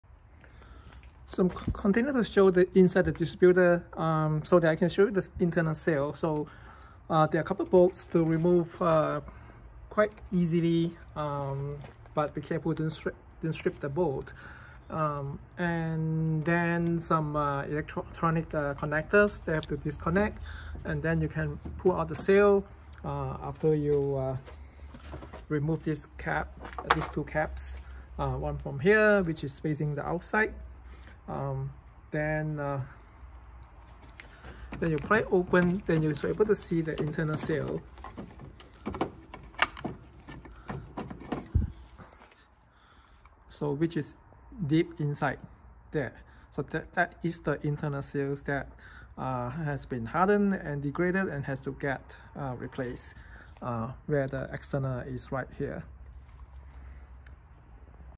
Honda/Acura Integra Spark Distributor Seal Replacement 3 of 3
Spark Distributor seal - outer o-ring and inner seal replacement